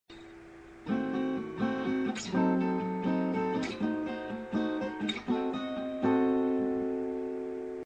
Adim7 progression
This is Adim7 but at each step with rotate voicing.

Chords (E.A.D.G.B.e):
x.0.1.2.1.2
x.0.4.5.4.5
x.0.7.8.7.8
x.0.10.11.10.11

x means don't play that string.